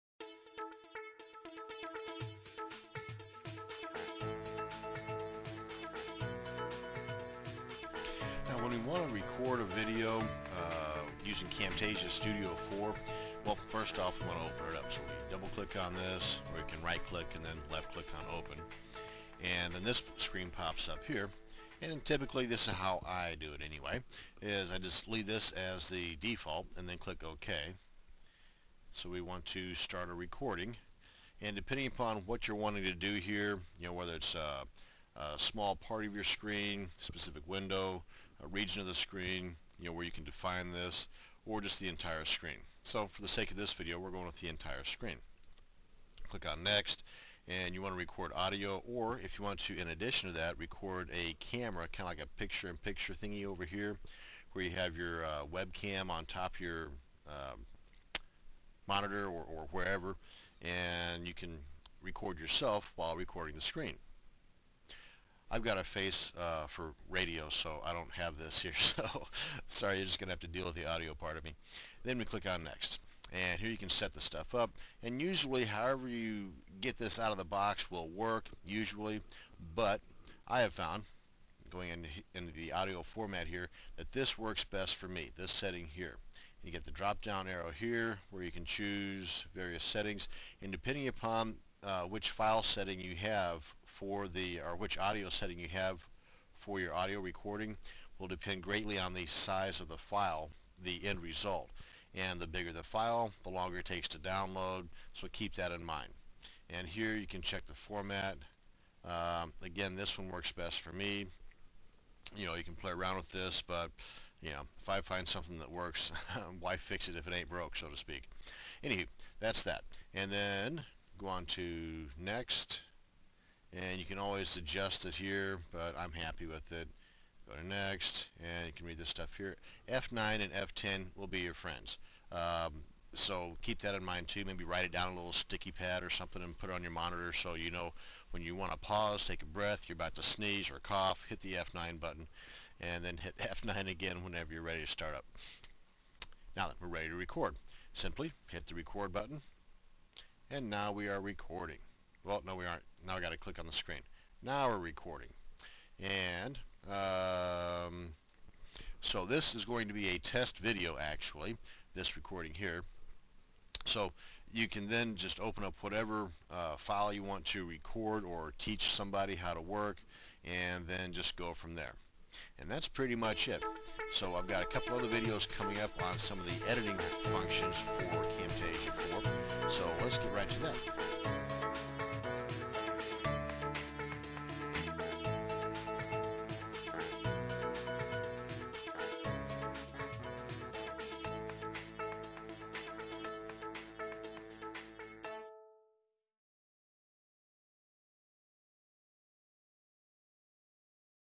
How To Setup Your Recording In Camtasia Studio
In this video Mr Internet Video shows you how to setup your recording in camtasia studio.